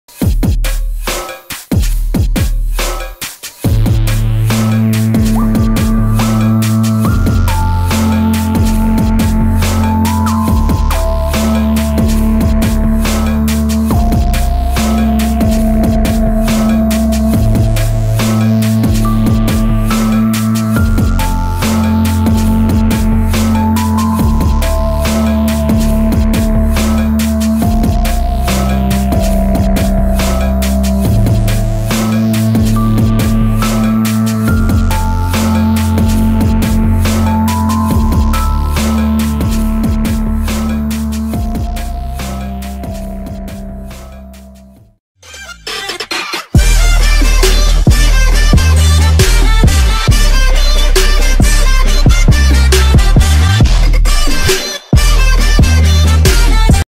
Ladybird beetle predating(feeding)on aphid | Predation & Biological control
Here, Ladybird beetle is a predator of aphid thus acting as a good biological control agent.
ladybird also acts as a predator of aphids,jassids,melay bugs etc.